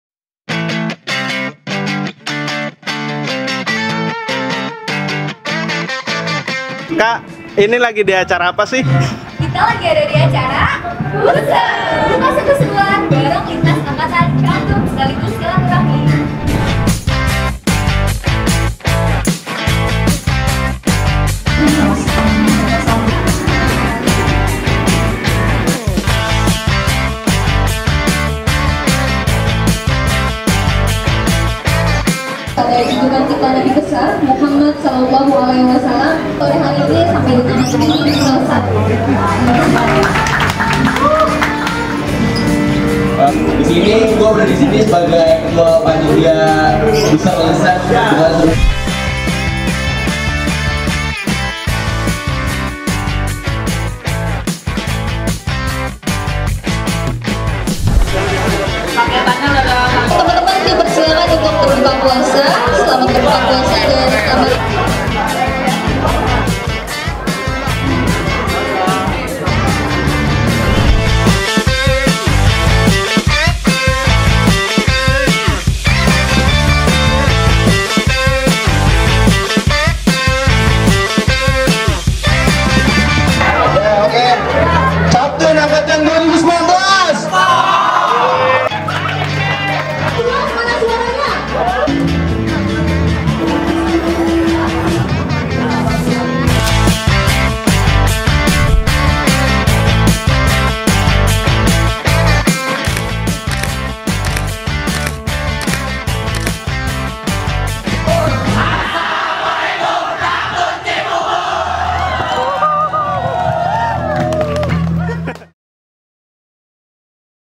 DOKUMENTASI VIDEO BUKBER ALUMNI SMKN 52 JAKARTA
Acara Buka Bersama Seru - seruan Lintas Angkatan. Alumni SMK N 52 JAKARTA ( Chaptoen Cibubur ) Berbagai Angkatan

Thanks To :
- Alumni SMK N 52 JAKARTA

yang sudah mempercayai kami buat mendokumentasikan acara Buka Bersama.

Information For Job & Bussines inquiry